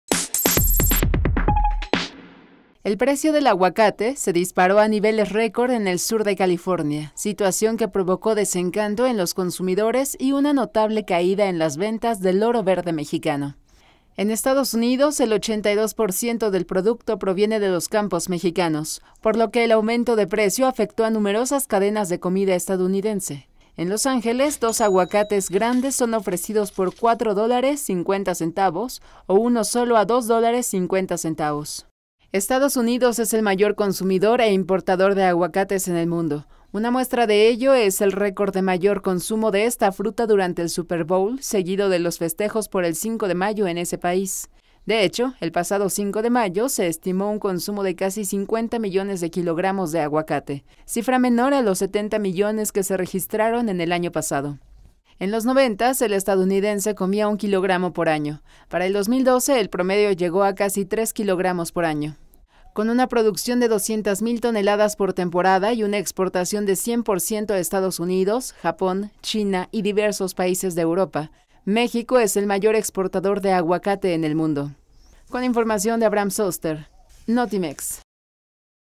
Oro verde, riqueza mexicana a la baja en Estados Unidos
El precio del aguacate se disparó a niveles récord en el sur de California, lo que causó bajas en las ventas